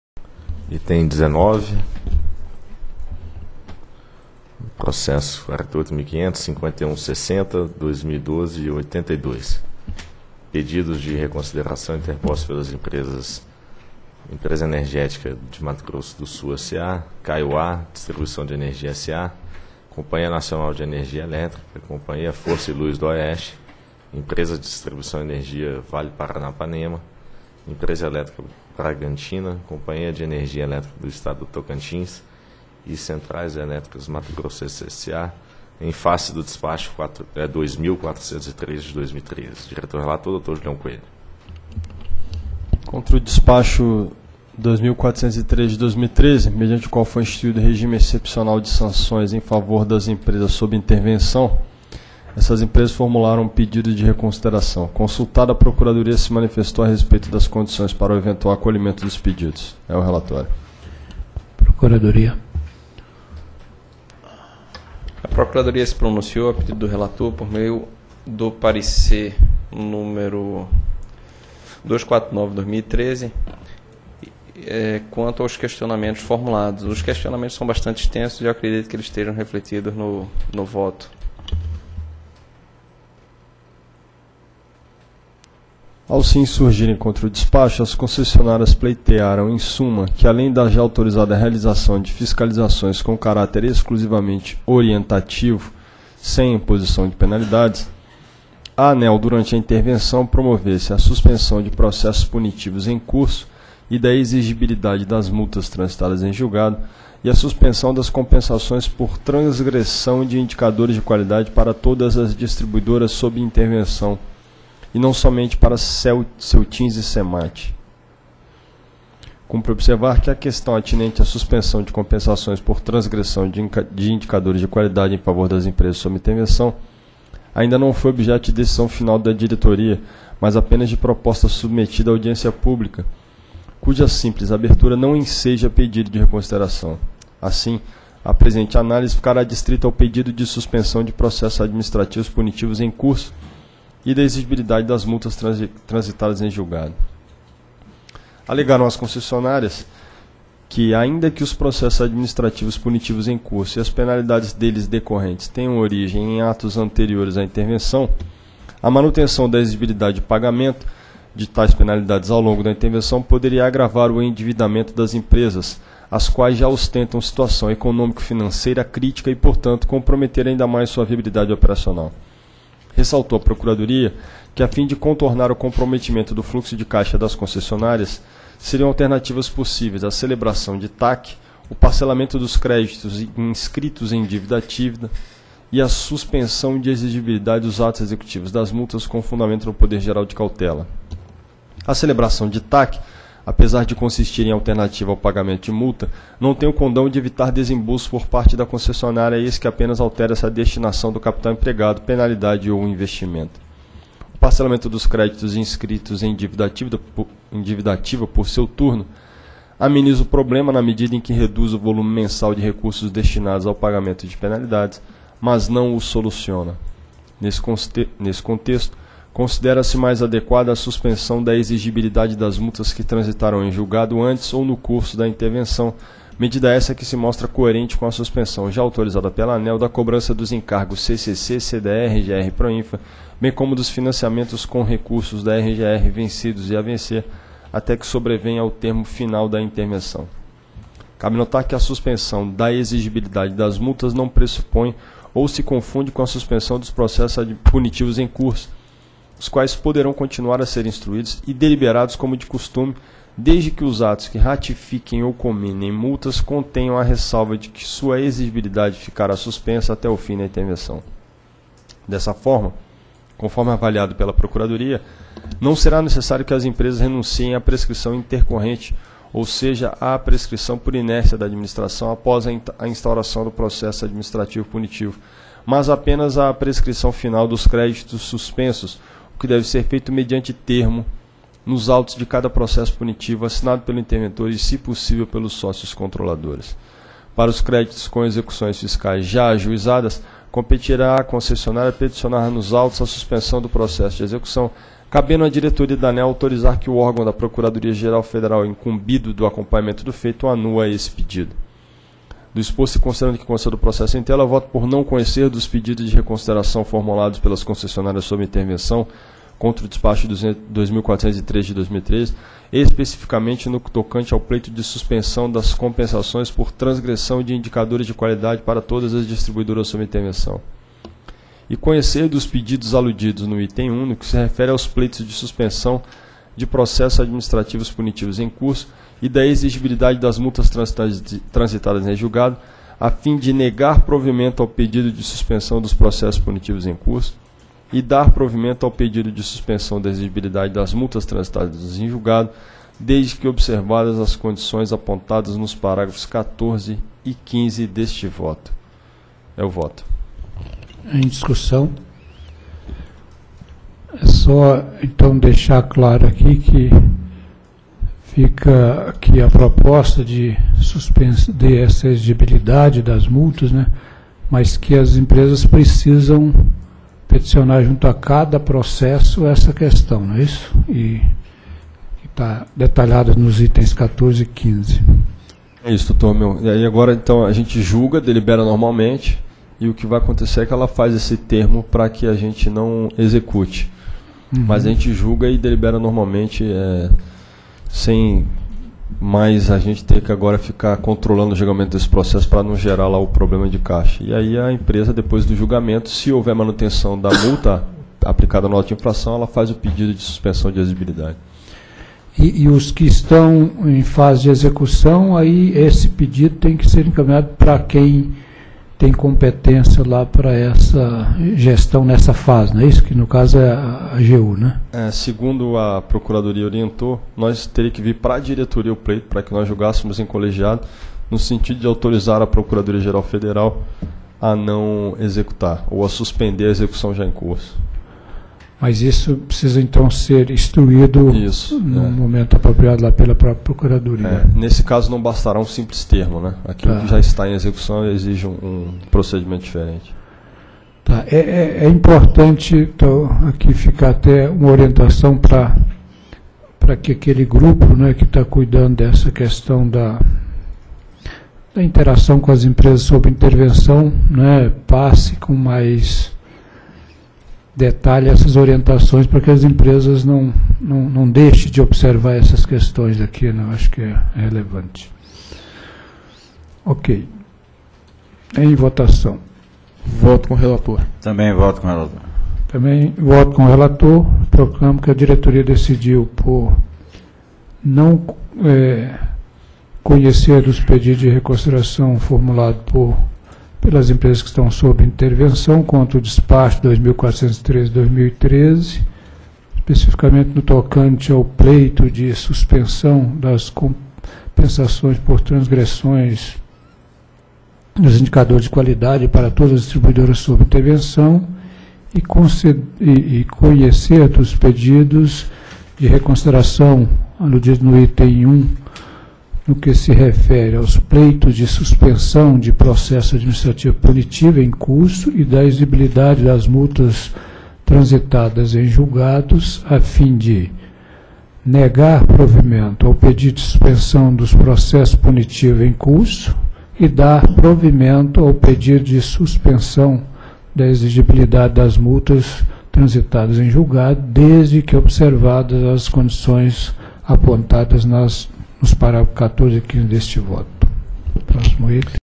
Item 19 - 17ª Reunião Pública Ordinária ANEEL 14/05/2013
Item 19. Processo: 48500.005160/2012-82. Assunto: Pedidos de Reconsideração interpostos pelas empresas Empresa Energética de Mato Grosso do Sul S.A. -- Enersul, Caiuá Distribuição de Energia S.A. -- Caiuá, Companhia Nacional de Energia Elétrica -- CNEE, Companhia Força e Luz do Oeste -- CFLO, Empresa de Distribuição Energia Vale Paranapanema S.A. -- EDEVP, Empresa Elétrica Bragantina S.A. -- EEB, Companhia de Energia Elétrica do Estado do Tocantins -- Celtins e Centrais Elétricas Matogrossenses S.A. -- Cemat em face do Despacho nº 2.413/2013. Relator: Diretor Julião Silveira Coelho.